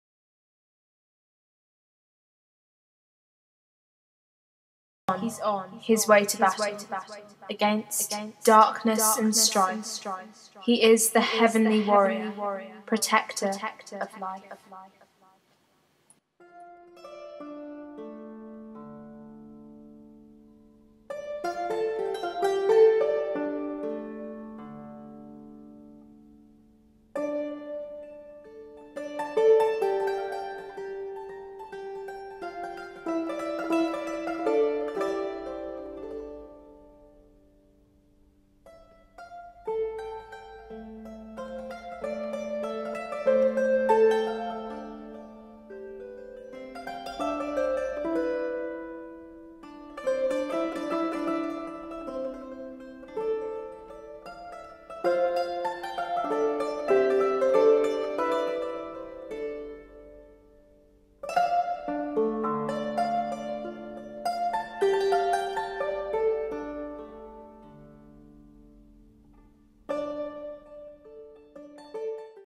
Arch : Trailer
TURN THE QUALITY UP TO 1080

Arch choreographed by Ruby Phelan

Arch is a dance about a girls spiritual journey when she summons four angels to guide her.

My choreography is being performed in the Brighton Spiegeltent on Saturday 31st May at 5pm.

Thankyou for all your support hope to see you at the show !